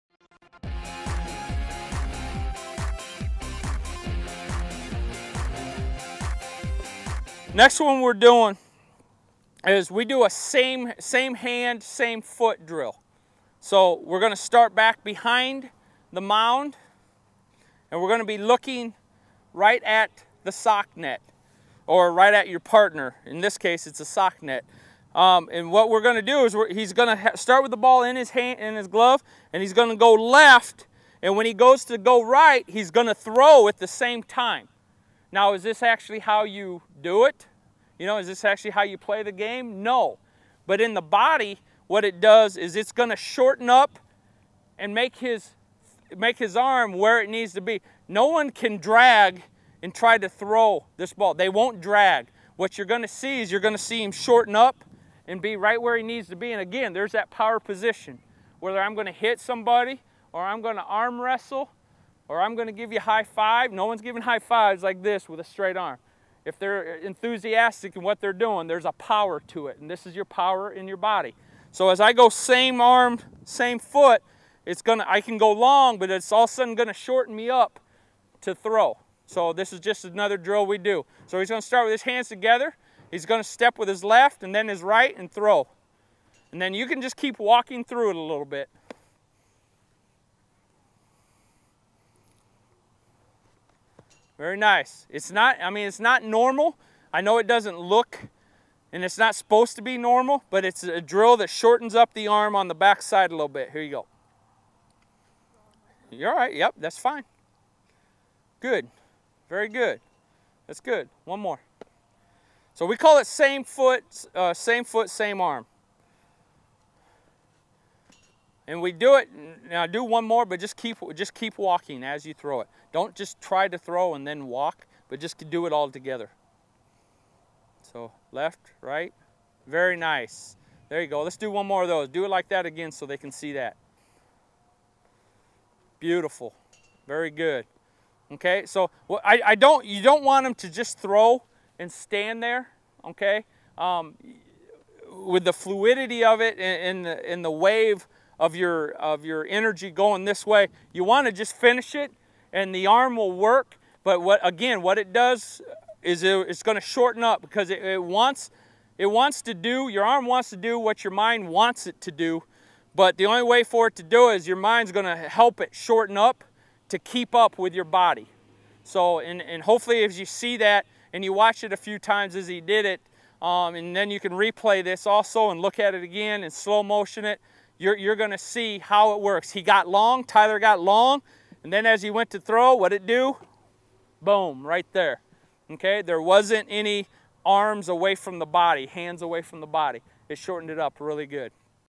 Teaching the Game: Pitching Skills and Drills for Youth Baseball
with Marc Rardin, Iowa Western Community College Head Coach; 2014 NJCAA Division I National Champions. Coach Rardin's philosophy is if you're throwing strikes, the other team has to put the ball into play. The way for pitchers to throw more strikes is by building a consistent motion. Coach Rardin shares multiple drills that emphasize staying "connected" and generating power through the natural movement of the body.